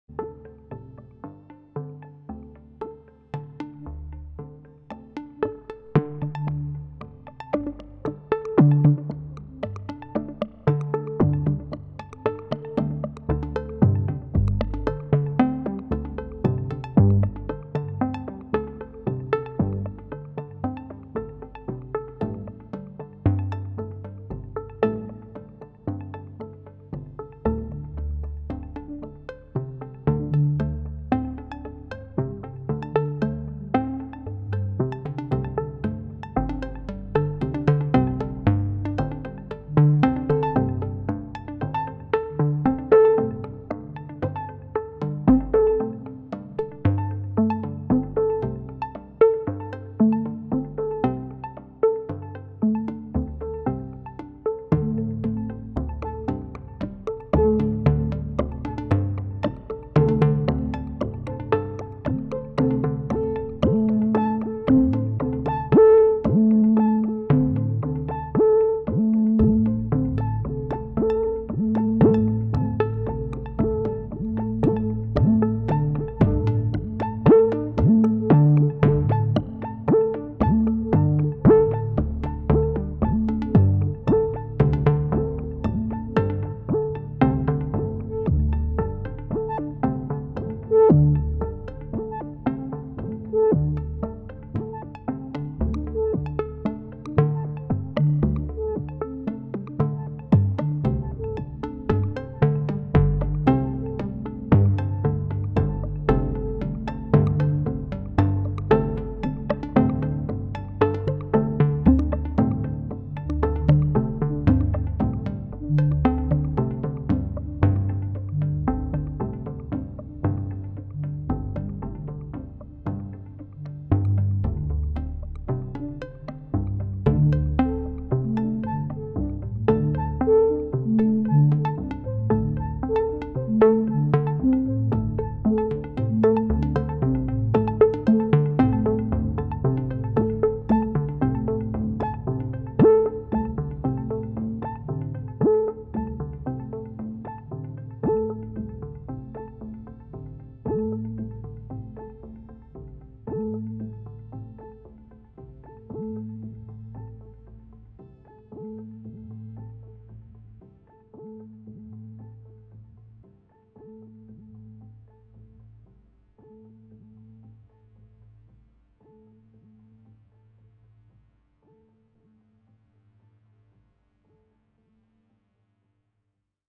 Verticals - Narrowing and animating Magneto's wide stereo field using Cold Mac
Verticals eurorack patch (largely generative).

Strymon Magneto can create beautiful wide stereo fields. I am using Cold Mac to narrow it slightly and a slow LFO to animate it further. I recommend listening to this one with headphones.

Lots of additional modulation using all that Batumi and math have to offer.

Tallin and Magneto both are overdriven making it all extra warm.

Cold Mac is doing double duty providing some analogue logic to gate some triggers.